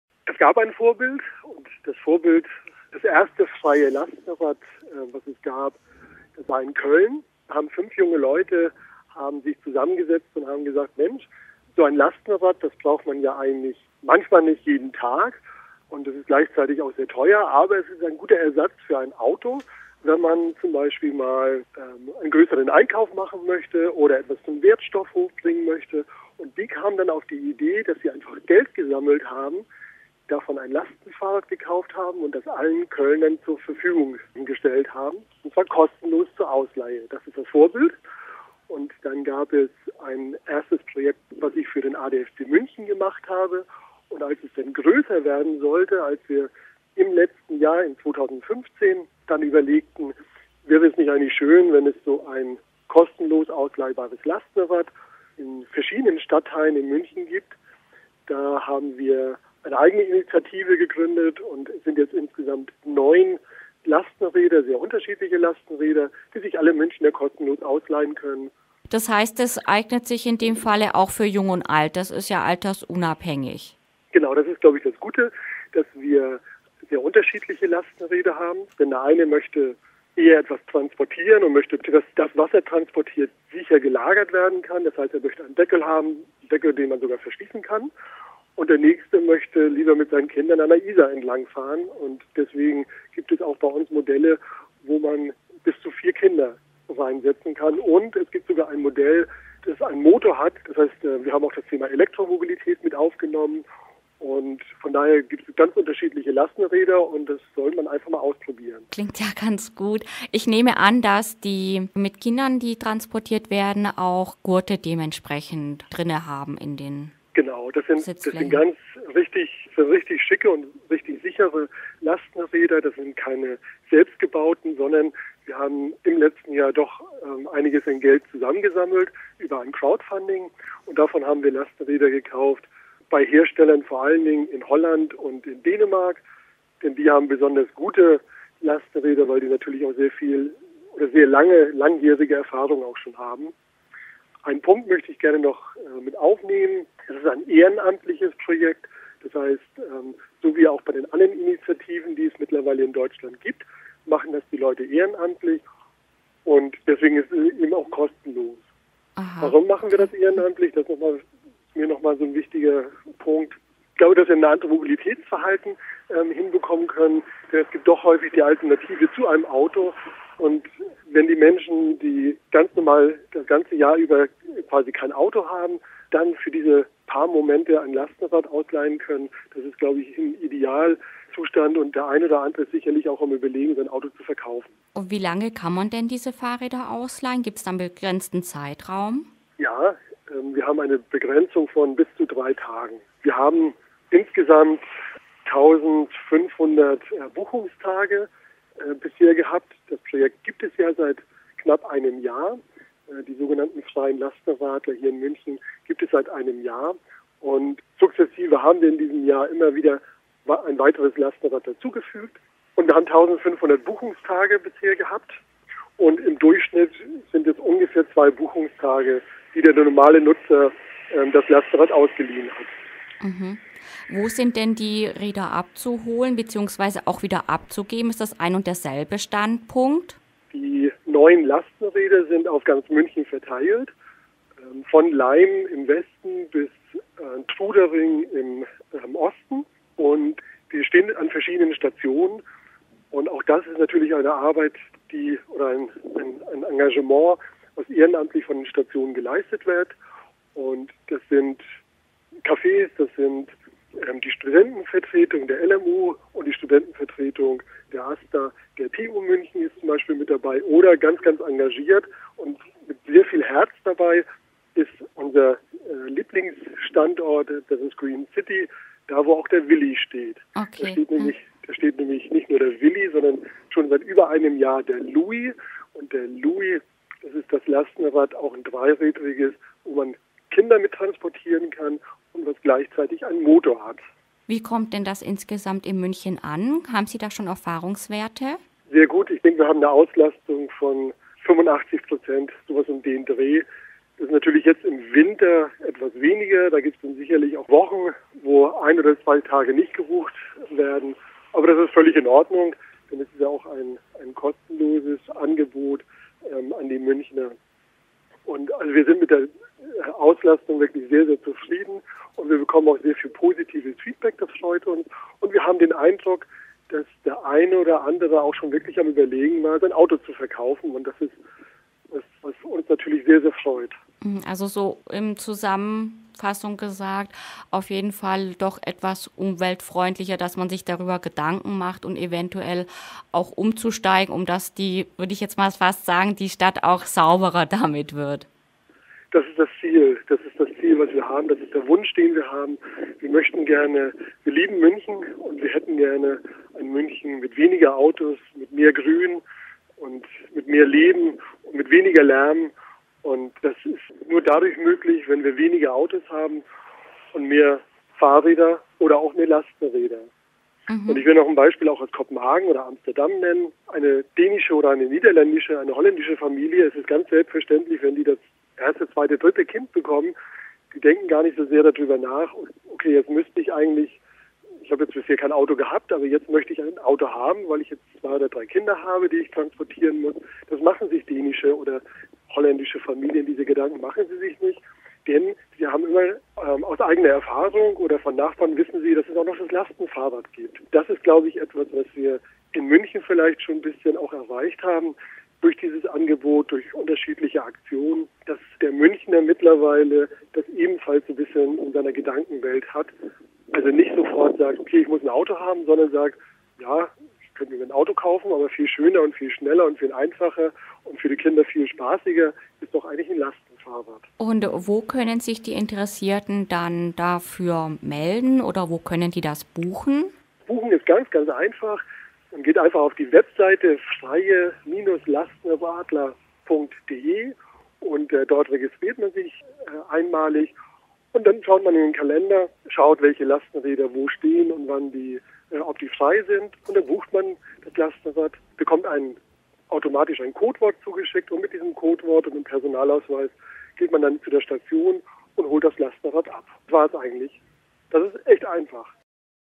Umdenken in der Mobilität - Kostenlose Lastenfahrräder in München (Radio Lora München)
Creative-Commons
Nichtkommerziell, Bearbeitung erlaubt, Weitergabe unter gleicher Lizenz erwünscht.
Autorin: Miriam Scharpf
Radio Lora München
Antikrieg TV